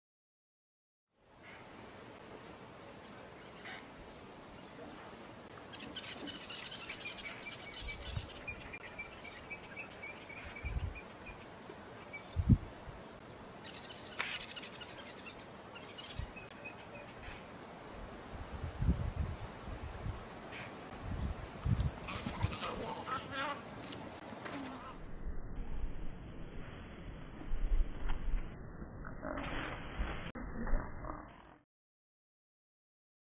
STEALTH + SPEED = KILL
The Emsagweni female leopardess lay patiently in ambush. It took about 20mins for the steenbok to unwittingly wander within striking distance. And when it did she struck like lightening!

MalaMala is the oldest and largest private game reserve bordering the Kruger National Park.